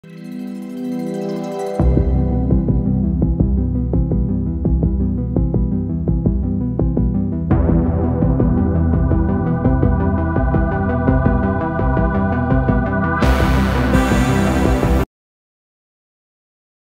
Painting of Mike…. Finn Wolfhard with watercolors !! oddly satisfying
Painting of Finn Wolfhard as Mike from stranger things (Netflix) with watercolors …Finn Wolfhard Fanart